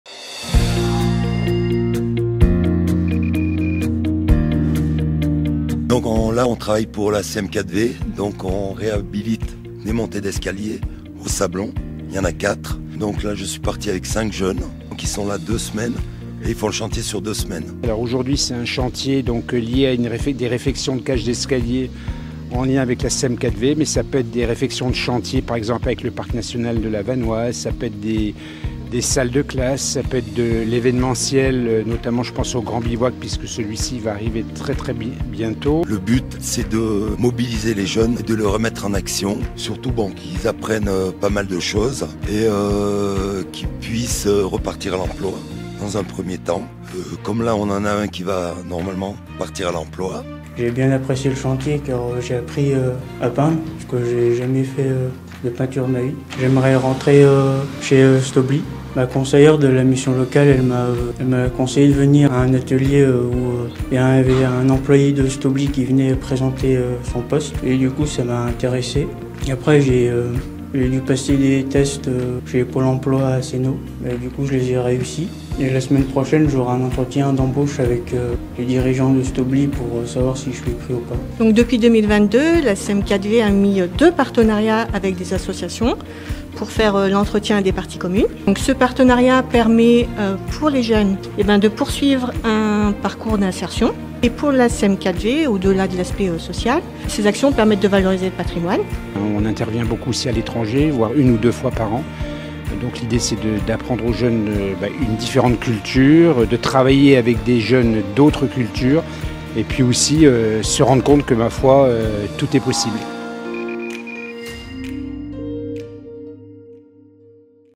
Ugine - Les jeunes de la Mission Locale Jeunes en chantier !
Solidarité - Les jeunes de la Mission Locale Jeunes en chantier !

Ce jeudi 14 septembre, 5 jeunes de la mission locale jeune d’Albertville se sont mobilisés afin de réaliser des travaux afin de venir en aide à la société d’économie mixte des 4 vallées durant 2 semaines.
Le but était de réhabiliter des montées d’escaliers avec l’aide de leur encadreur technique, Jean-Enzo BOUGUETTOUCHA mais aussi de remettre ces jeunes en actions afin de leur permettre, par la suite, de pouvoir trouver plus facilement un emploi.

Plus d’informations en vidéo avec les paroles de Jean-François BRUGNON - Président de la Mission Locale Jeunes ainsi que Karinne QUETIER - Responsable du patrimoine et du développement de la SEM4V.